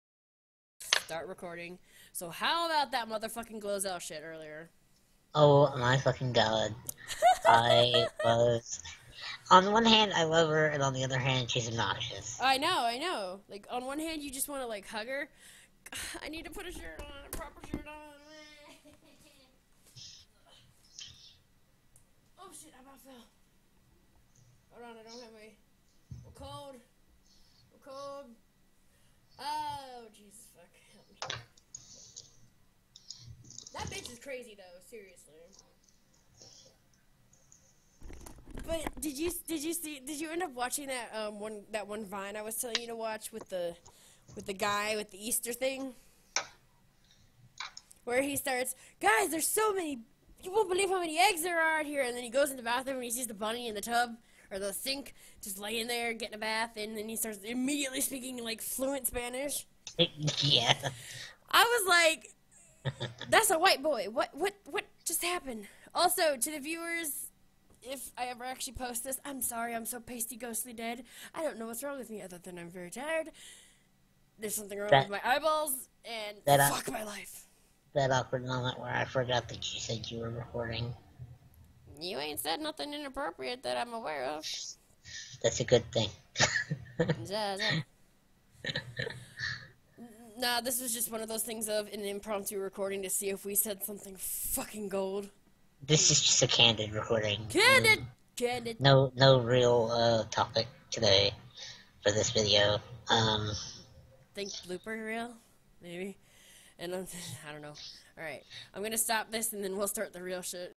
Candid recording
In which Phenix and Glytchy begin it all. Our names are misspelled intentionally. Let It GO.

We just decided to record a random convo we were having. So yeah.